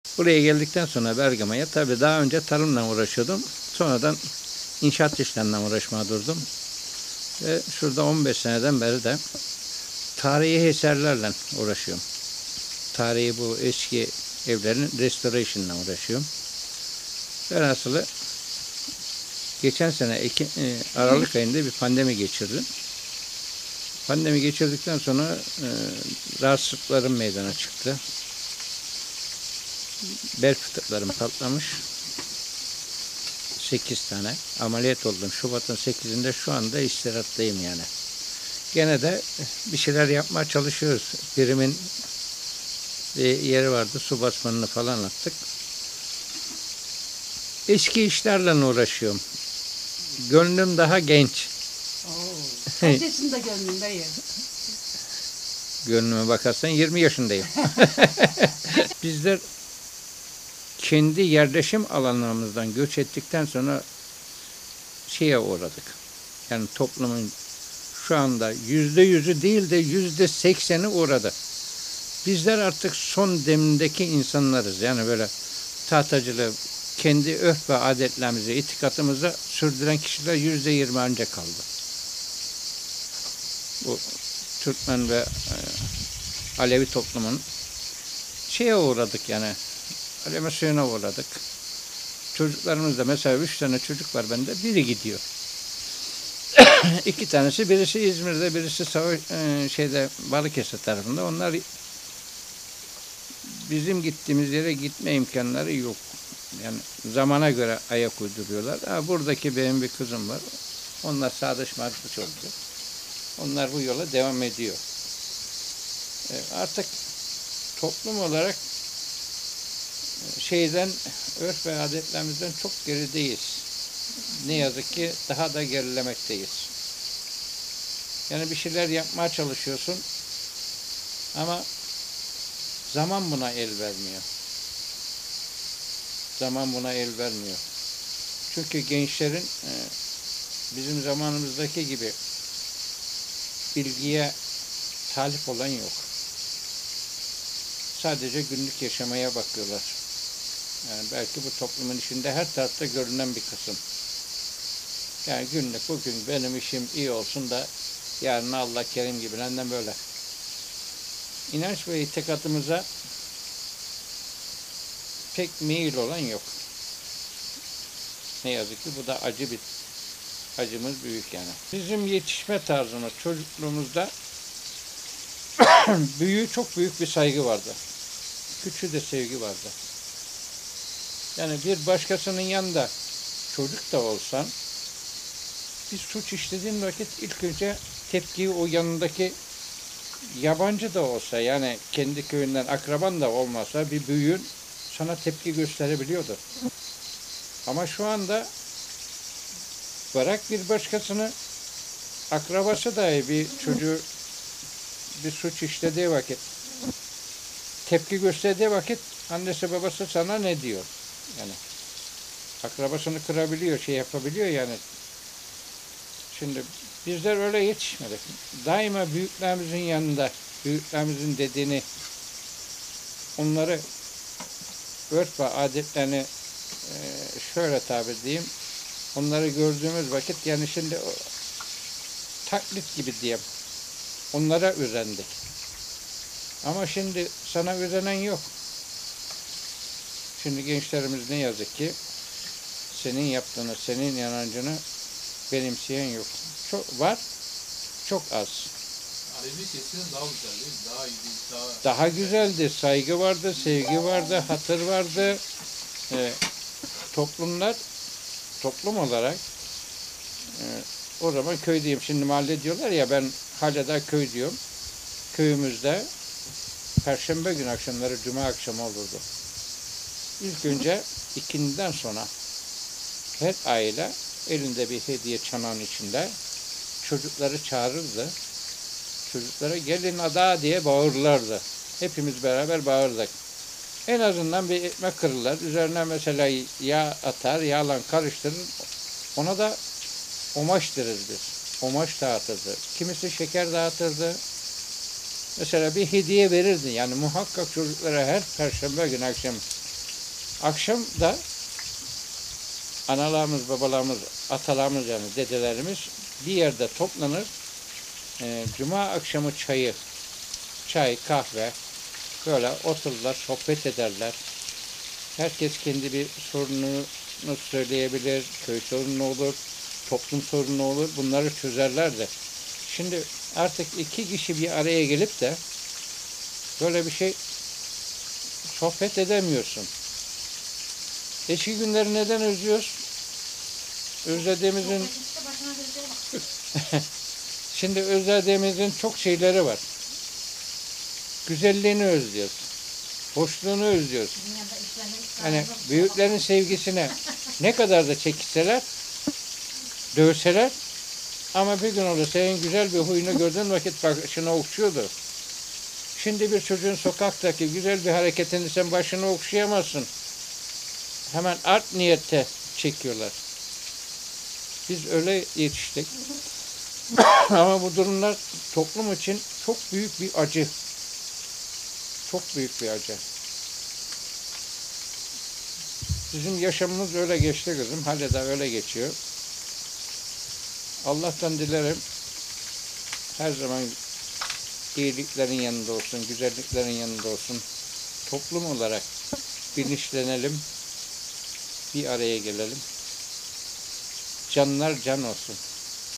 ‘Tahtacı Aleviler olarak inancımızdan koptuk, eskilerin güzelliğini özlüyoruz’
PİRHA- İzmir Bergama’ya bağlı Kapukaya köyünde yaşayan İsmail Sevik, Tahtacı Alevilerin toplumsal yaşam alanlarından göç etmesi sonrası şehirleşme ile birlikte inançlarının da geriye düştüğüne vurgu yaparak, “Bizler kendi yerleşim alanlarımızdan göç ettikten sonra asimilasyona uğradık. Bizler artık son demdeki insanlarız. Tahtacıların örf adetlerini ve inancını sürdüren son kişileriz. İnanç ve itikatimize meyli olan yok” diye konuştu.